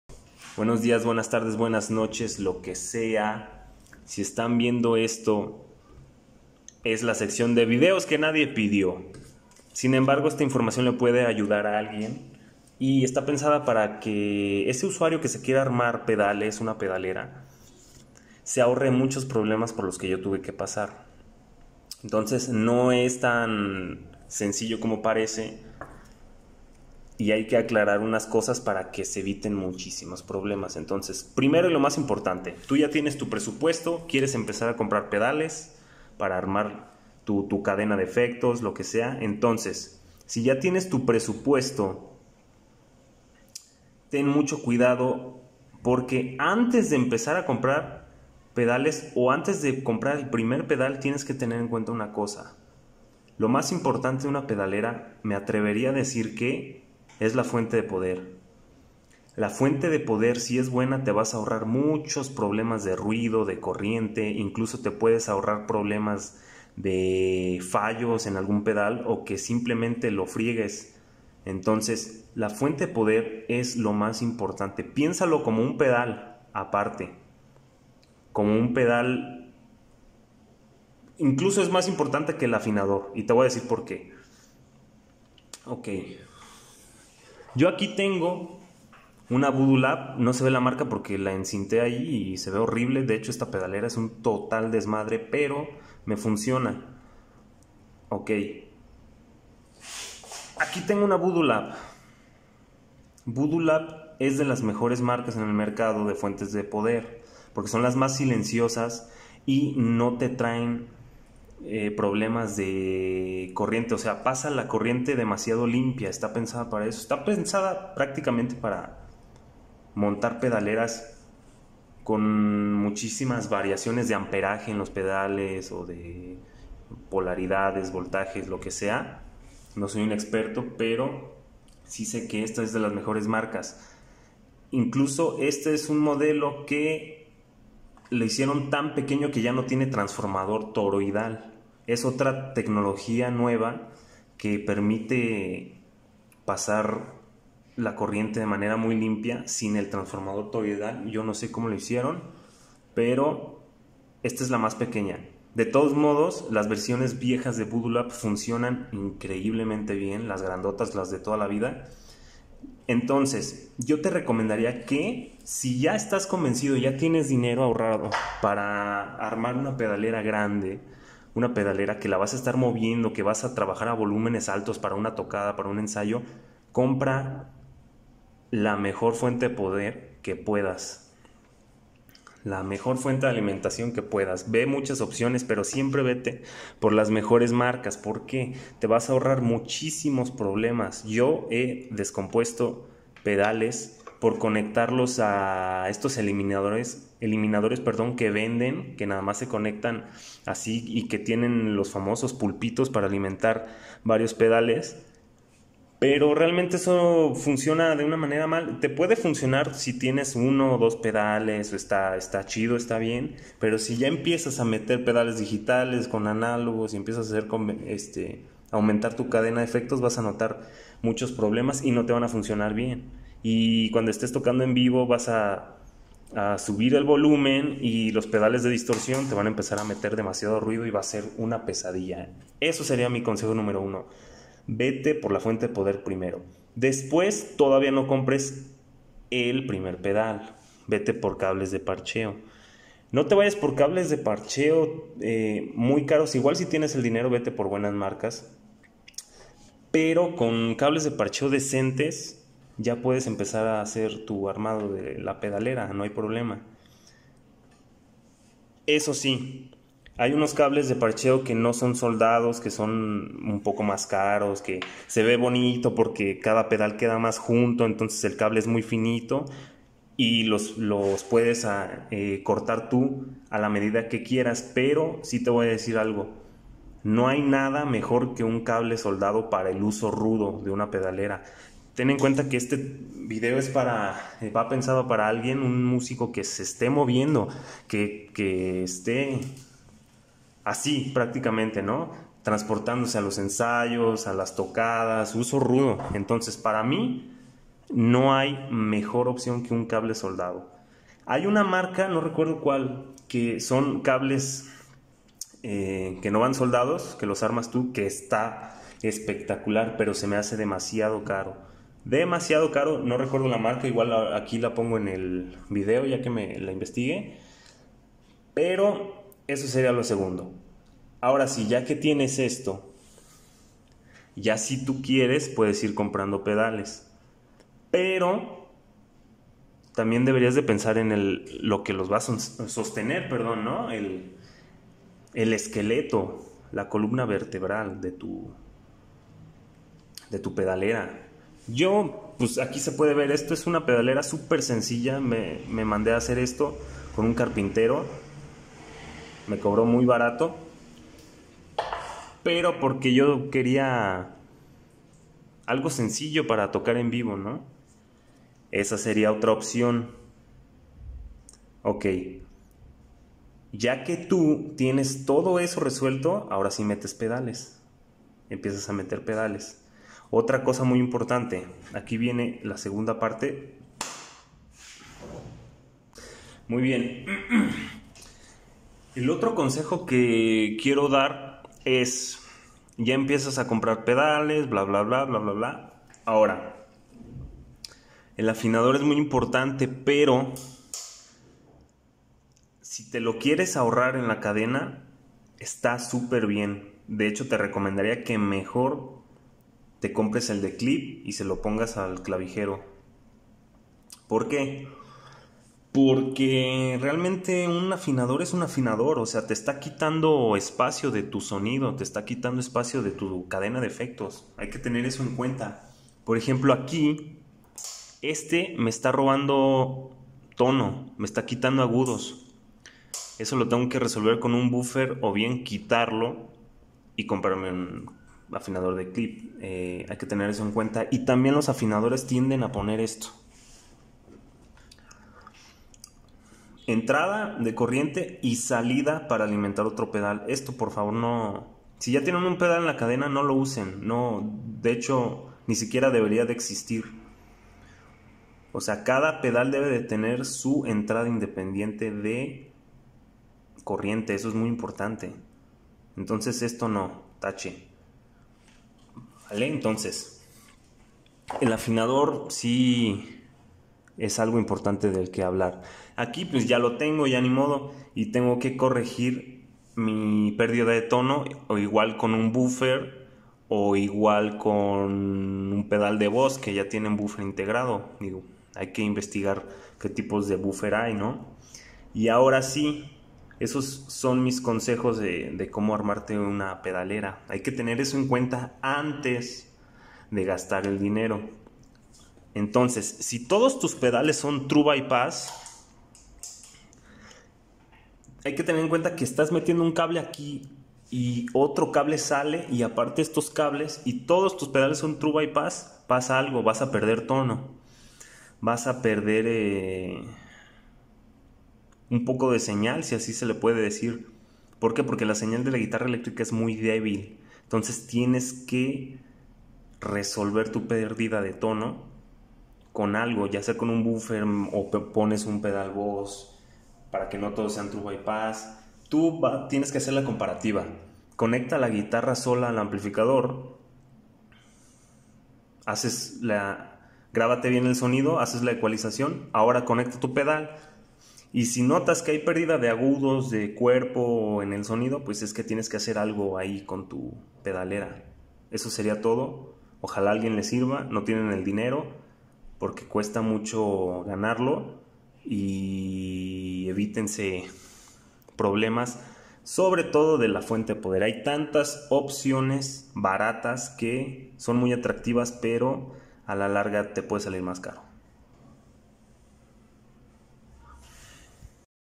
Consejos para armar una pedalera
toda la info que es un recopilado de otros video de gente que ya lleva muchísimos años de experiencia